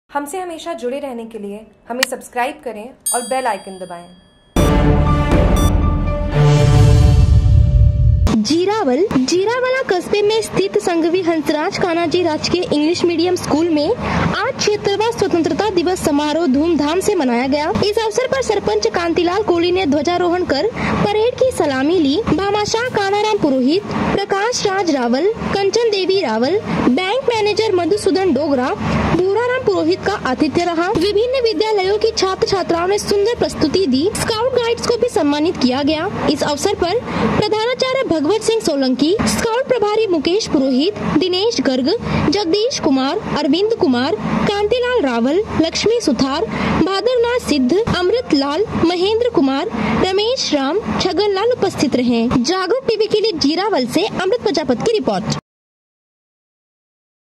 जीरावल : राजकीय इंग्लिश मीडियम स्कूल में आज 76 वां स्वतन्त्रता दिवस समारोह धूमधाम से मनाया गया
जीरावला कस्बे में स्थित संघवी हंसराज कानाजी राजकीय इंग्लिश मीडियम  स्कूल में आज 76 वां स्वतन्त्रता दिवस समारोह धूमधाम से मनाया गया। इस अवसर पर सरपंच कांतिलाल कोली ने ध्वजारोहण कर परेड की सलामी ली। भामाशाह कानाराम पुरोहित , प्रकाश राज रावल, कंचन देवी रावल , बैंक मैनेजर मधुसूदन डोगरा , भूराराम पुरोहित का आतिथ्य रहा। विभिन्न विद्यालयों के छात्र छात्राओं ने सुंदर प्रस्तुति दी। स्काउट गाइड्स को भी सम्मानित किया गया।इस अवसर पर प्रधानाचार्य भगवत सिंह सोलंकी,स्काउट प्रभारी  मुकेश पुरोहित, दिनेश गर्ग,  जगदीश कुमार, अरविंद कुमार , कांतिलाल रावल, लक्ष्मी सुथार, भादर नाथ सिद्ध, अमृत लाल, महेंद्र कुमार, रमेश राम , छगनलाल उपस्थित रहे